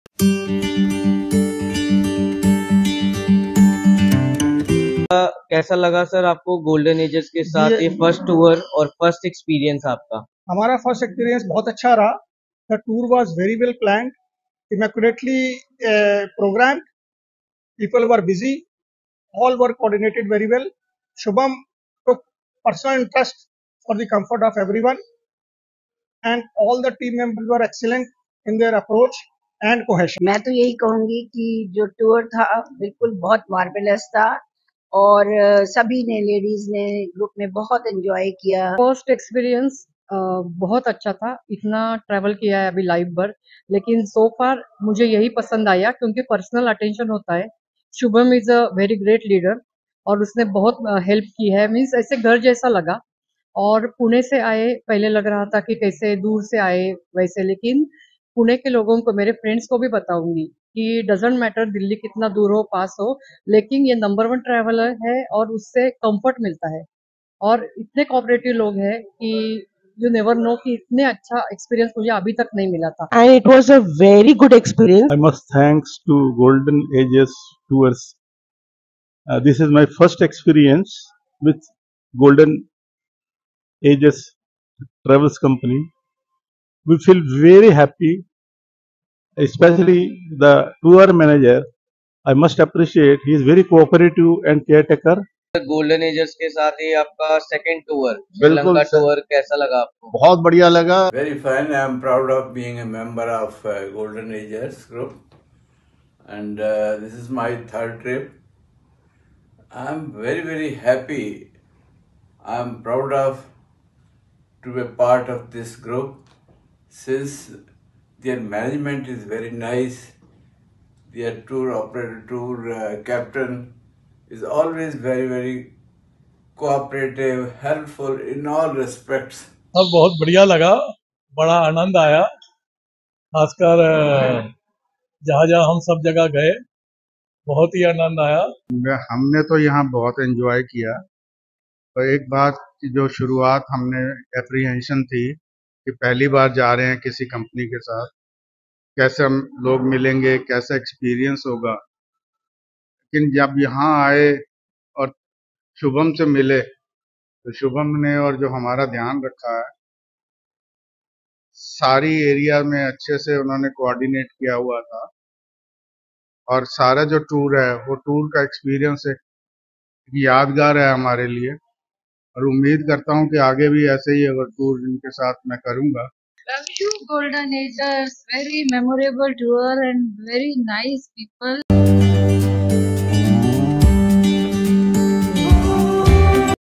[GoldenAgers.in] Customer Reviews Senior Citizen Sri Lanka Group Tour
Senior citizen Sri Lanka group tour customer reviews by all goldenAgers dubai tour traveler. All the goldenAgers traveler are happy with our best hotel accommodation with delicious food, River Boat Cruise, Turtle Hatchery, Temple of the Tooth Relic, Bentota, Kandy, Hanuman Temple, Nuwara Eliya, Seetha Amman Temple, Ashok Vatika, Colombo City Tour, Colombo, etc…, Sightseeing and personal care with goldenAgers Group assistance.

Book your seats for next Senior Citizen (50+ years) Tour of GoldenAgers.